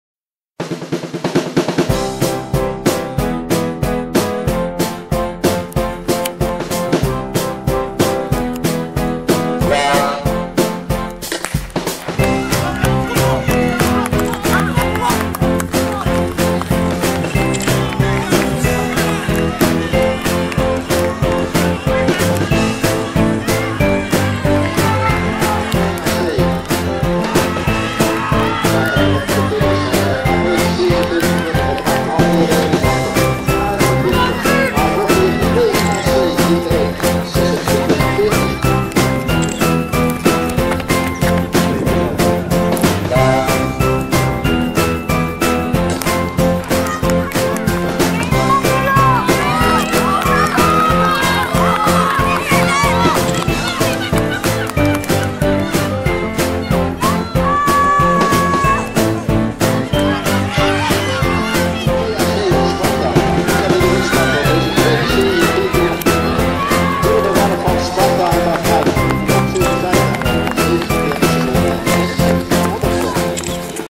Jongens C 4 x 100m series PAC Rotterdam CD Competitie Finale 13-09-2014
Jongens C 4 x 100m series CD Competitie Finale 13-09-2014 Atletiekbaan Schothorst, Amersfoort PAC Rotterdam Almere 81, AV Sparta AV40 AV Flevo Delta